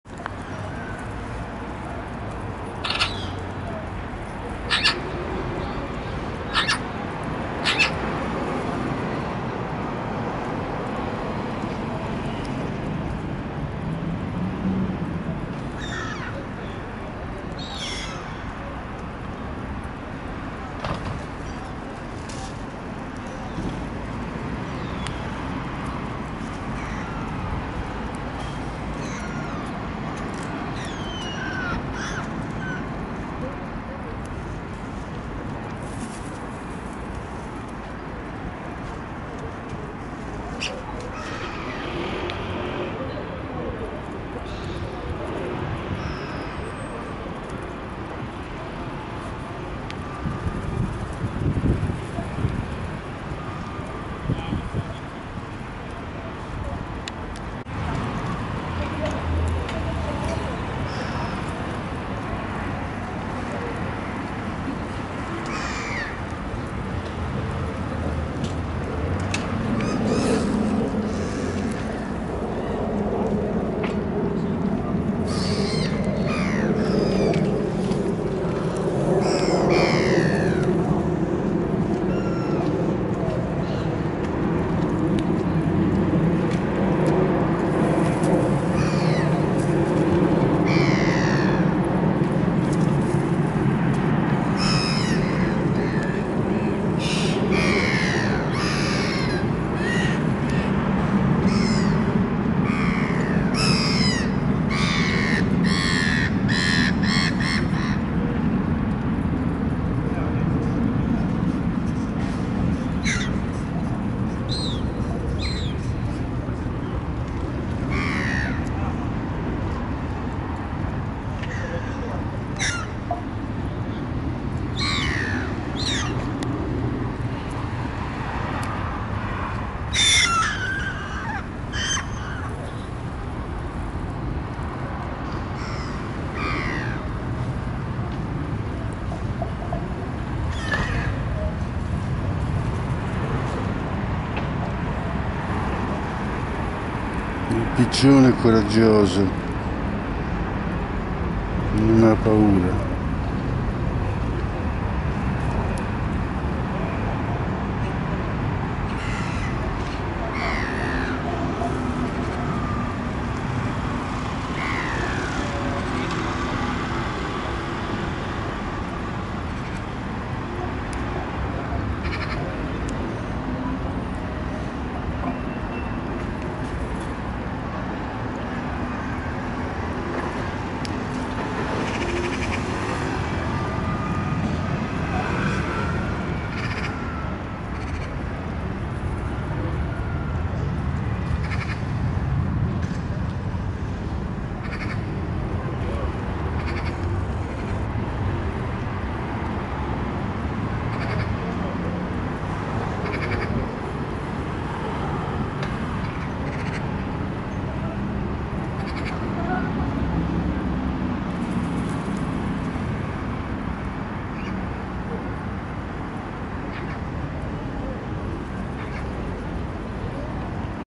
Filming Birds.
I love to film birds, pigeons are not afraid and do not fly away.
Filmando Uccelli.
Mi peace filmare uccelli, specilamente I colombi che non hanno paura e non volano via.